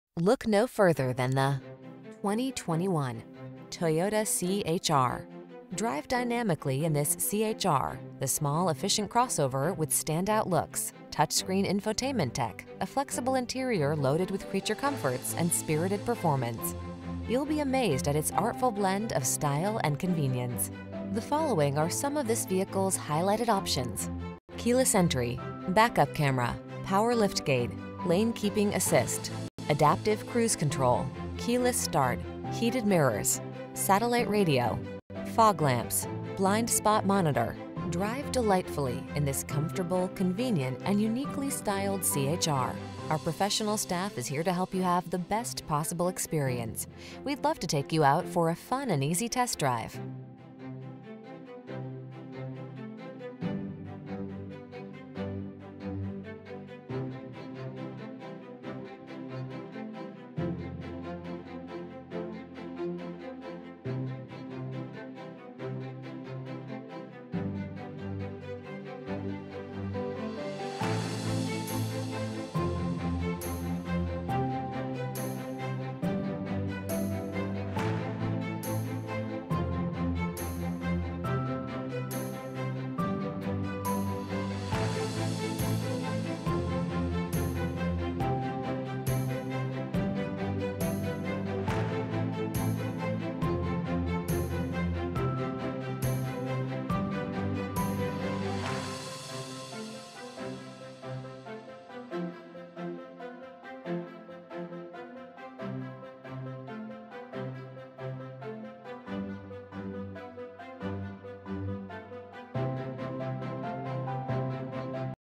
2021 Toyota C-HR Claremont, Ontario, Pasadena, Anaheim, CA 00212651
Blue Eclipse Metallic New 2021 Toyota C-HR available in Claremont, California at Claremont Toyota. Servicing the Ontario, Pasadena, Anaheim, CA area.

Claremont Toyota
601 Auto Center Drive

08x2 2021 Toyota C-HR Limited FWD CVT 2.0L I4 DOHC 16V27/31 City/Highway MPG